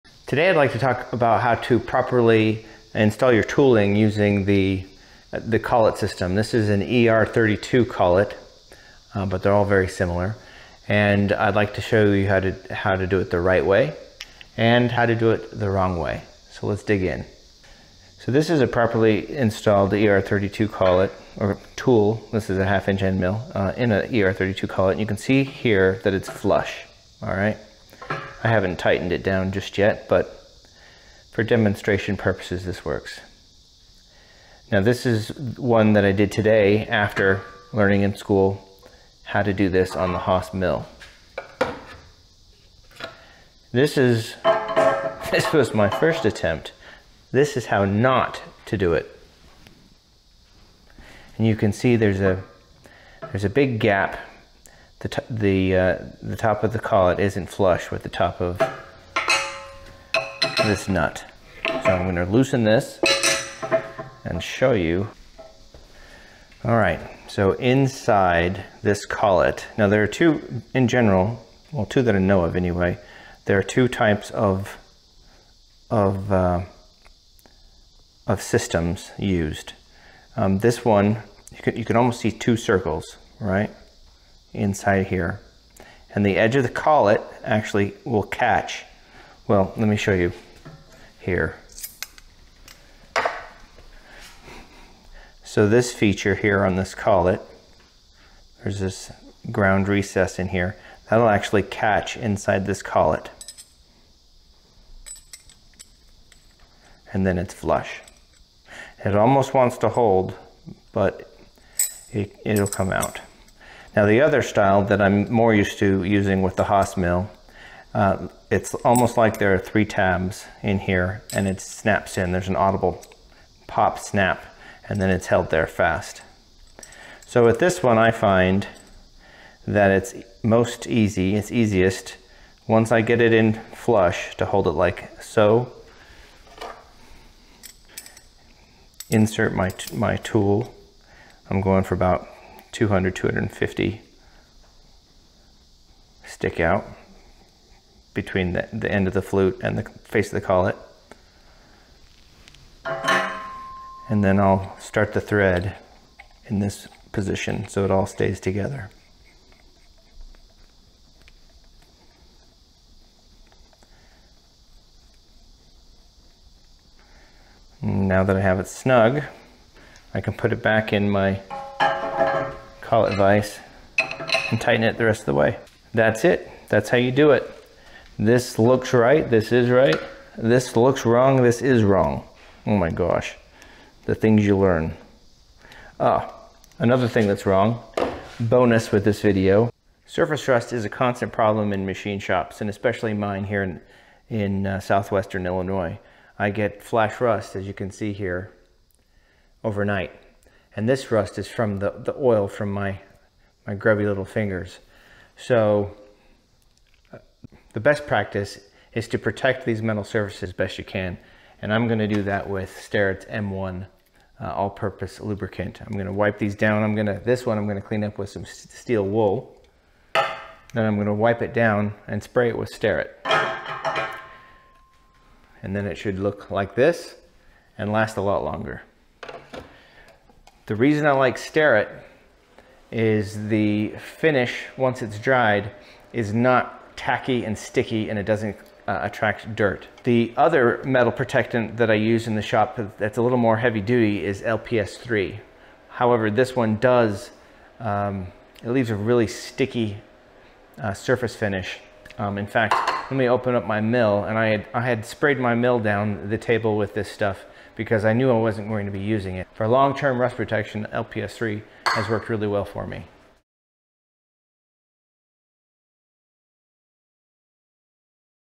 SYIL X7 Basics: How to Install a Tool in an ER32 Collet
In this video, I explain how to install a tool in an ER32 collet.

**This video was filmed in September of 2022.  I held off on publishing it until I had a bit more experience using the Syil**